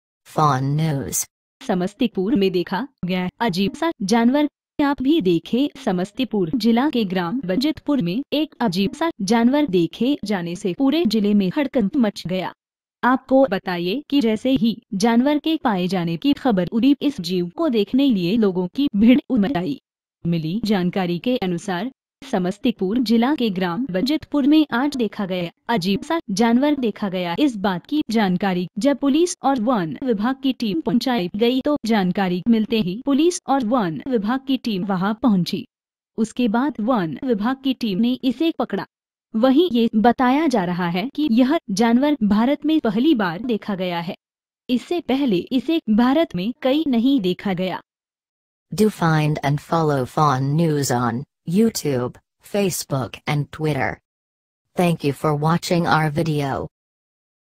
समस्तीपुर में देखा गया अजीब सा जानवर, आप भी देखें: FAWN NEWS
समस्तीपुर जिला के ग्राम बजीतपुर में एक अजीब सा जानवर देखे जाने से पूरे जिले में हड़कंप मच गया। आपको बताये कि जैसे ही जानवर के पाये जाने की ख़बर उडी इस जीव को देखने लिए लोगों की भीड़ उमड़ आई।
मिली जानकारी के अनुसार, समस्तीपुर जिला के ग्राम बजीतपुर मे आज देखा गया अजीब सा जानवर देखा गया इस बात की जानकारी जब पुलिस और वन विभाग की टीम पहुंचाई गयी तो जानकारी मिलते ही पुलिस और वन विभाग की टीम वहां पहुंची। उसके बाद वन विभाग की टीम ने इसे पकड़ा।
वहीं ये बताया जा रहा है कि यह जानवर भारत मे पहली बार देखा गया है। इससे पहले इसे भारत में कई नहीं देखा गया।